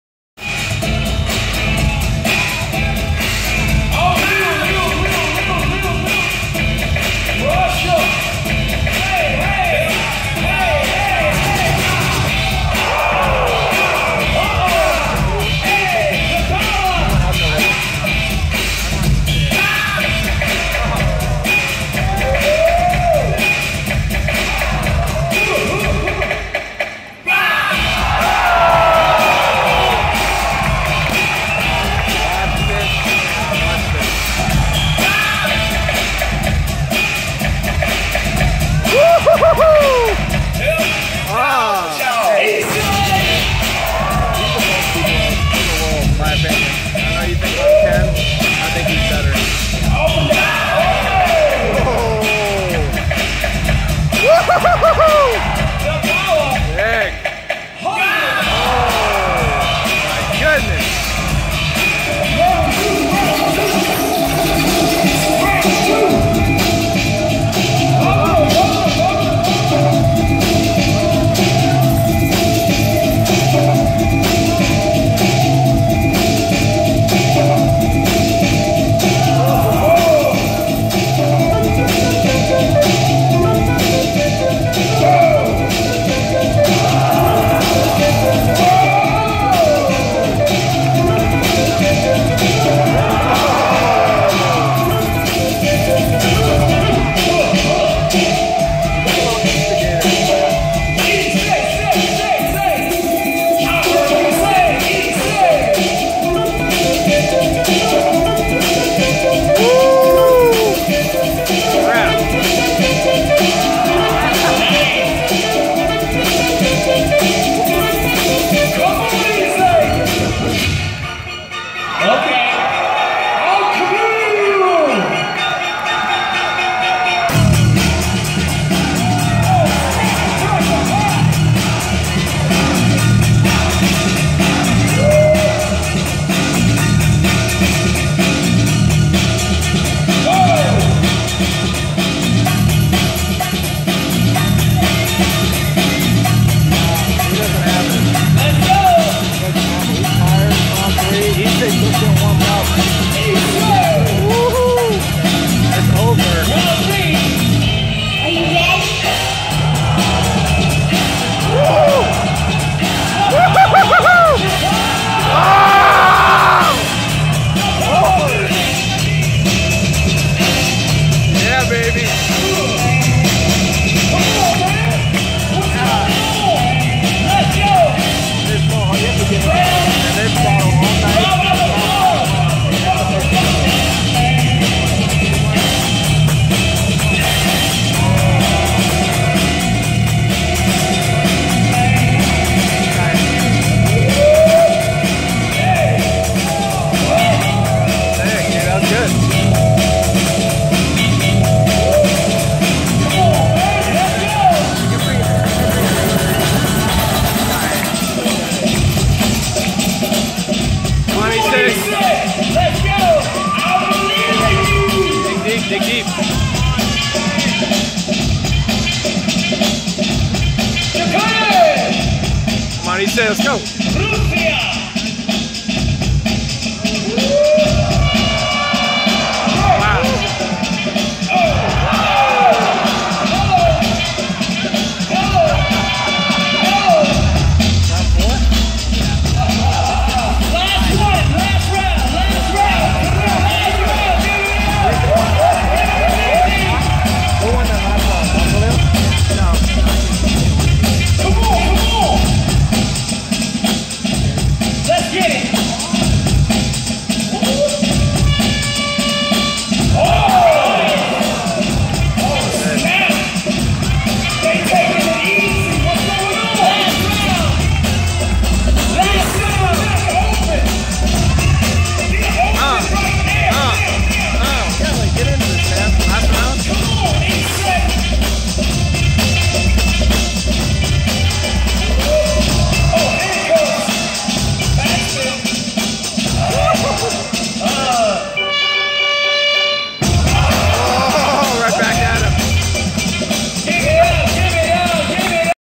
Issei vs Alkolil// Final Battle// Silverback Open 2017
Issei vs Alkolil Final Battle 2017

Winner: Issei
www. breakingforJesus.com